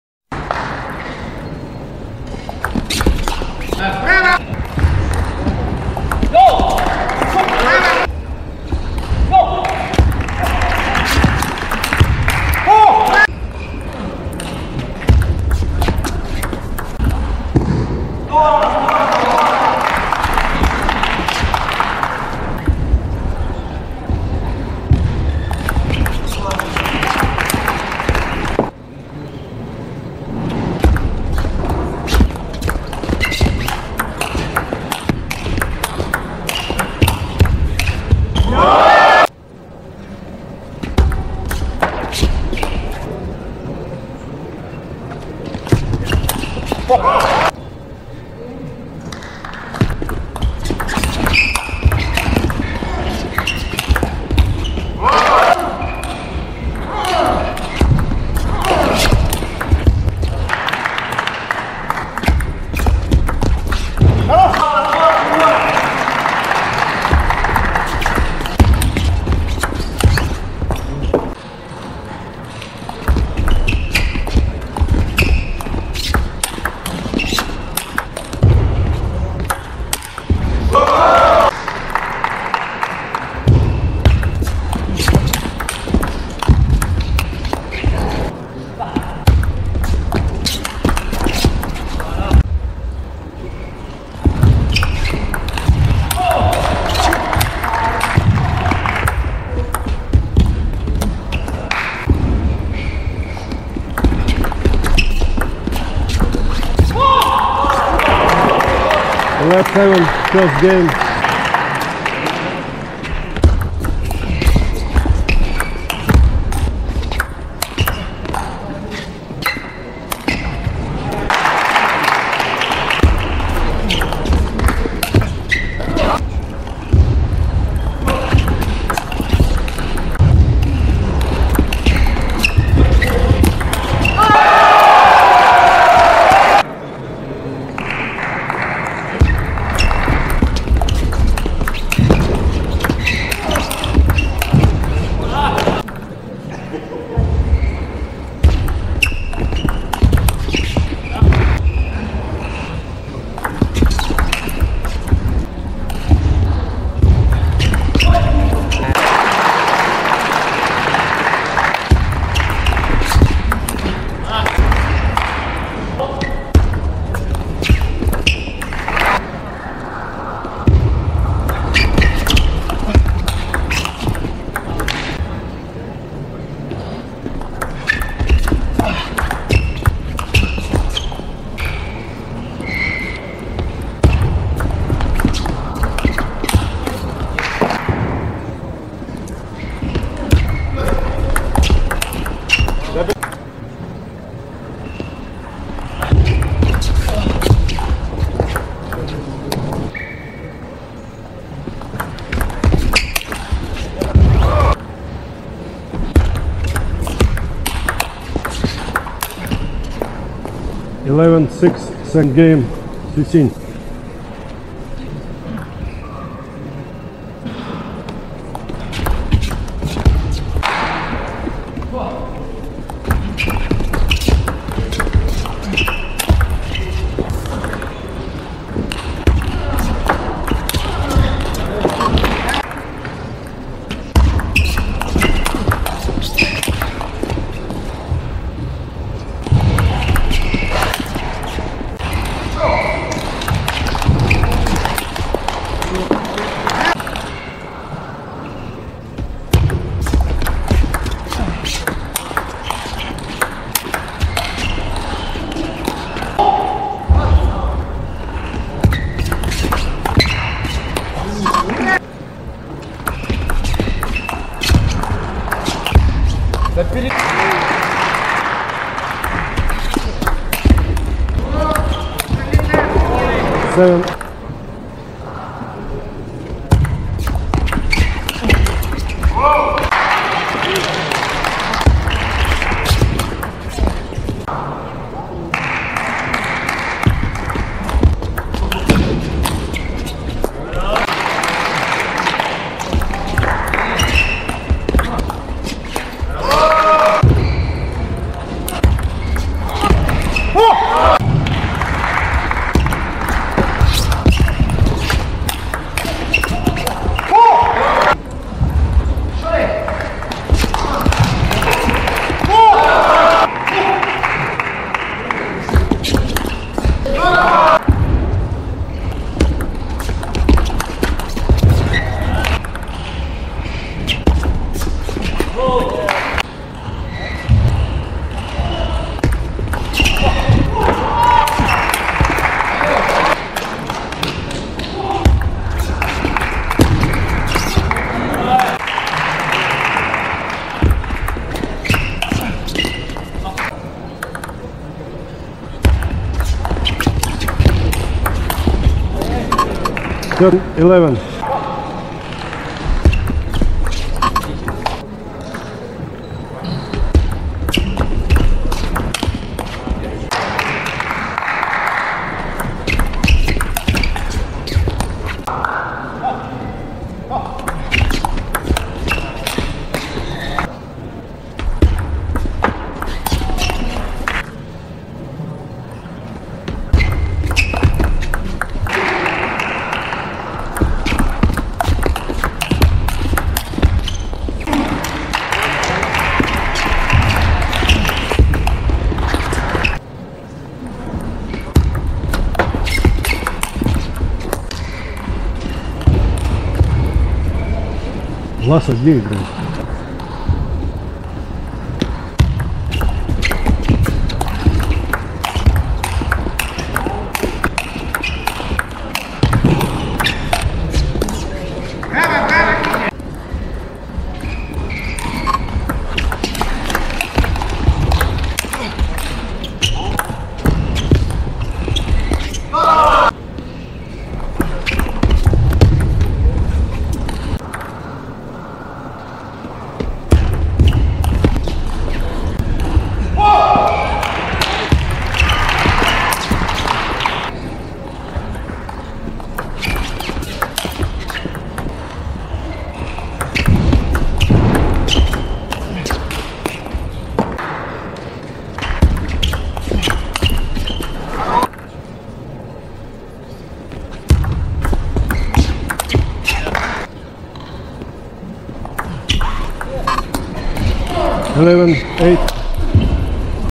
2012 Koltsovo Russia Open (ms-R32) XU Xin - VLASOV Grigory [Full/Short Form]
2012 ITTF World Tour, Koltsovo Russia Open,Sep 12-16, Ekaterinburg, RUS
-- Your Home For World-Class Competition Online - Free!
For Interviews, Videos, curiosity about the best TT players in the world